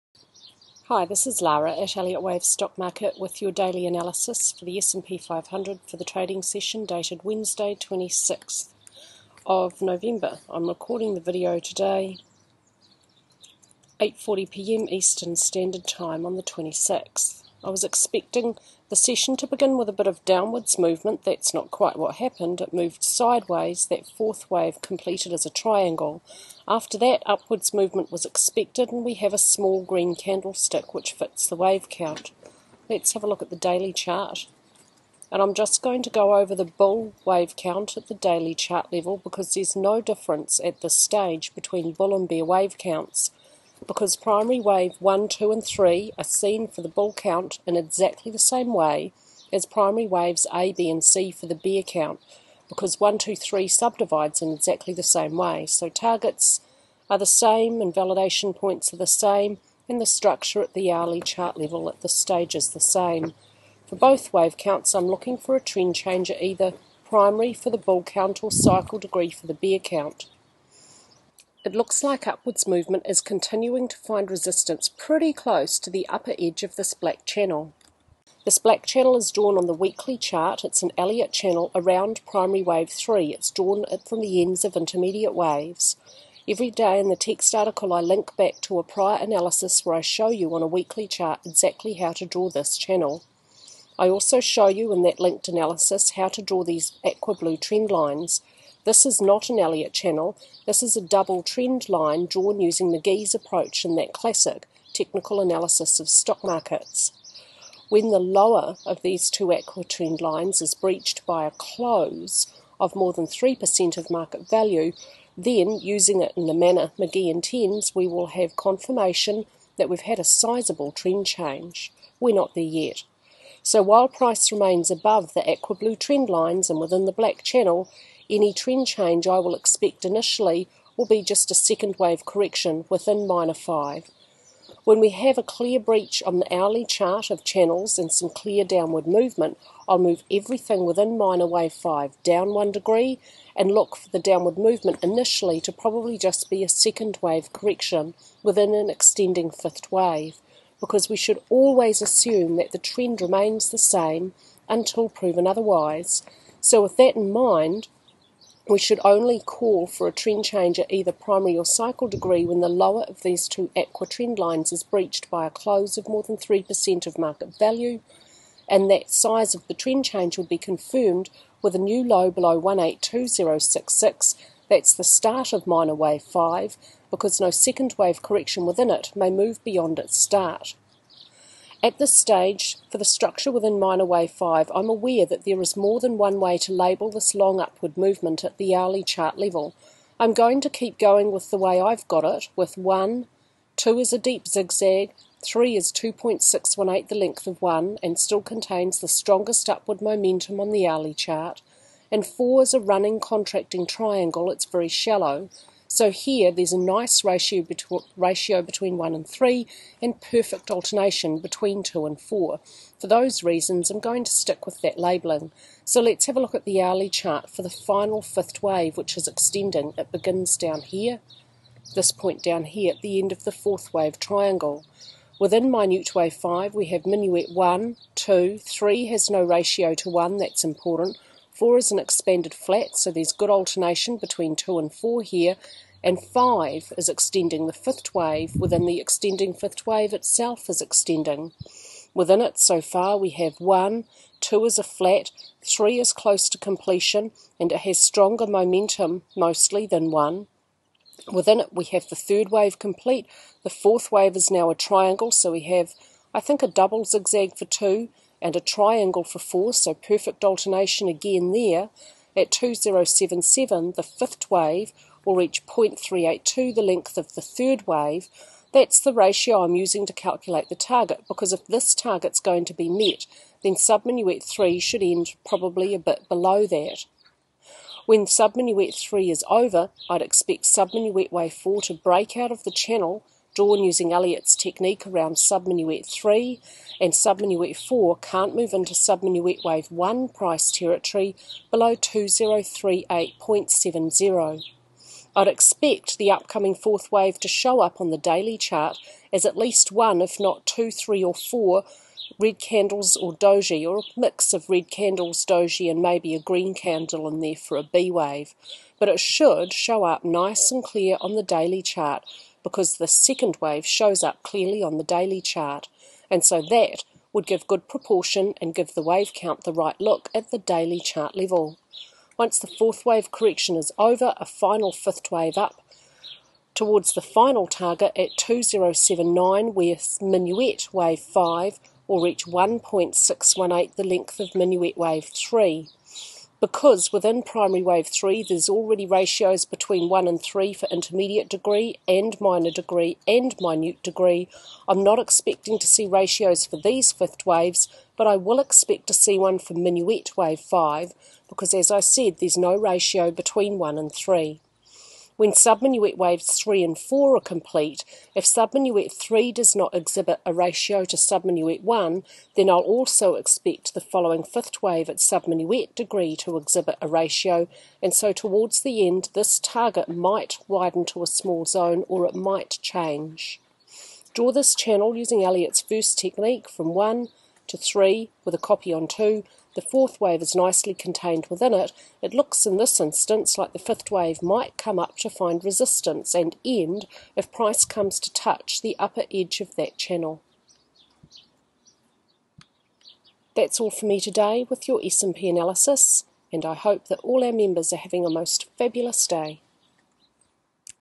S&P 500 Elliott Wave Technical Analysis - 26th November, 2014
Technical analysis of the S&P 500 cash index from a pure Elliott wave perspective after market close of 26th November, 2014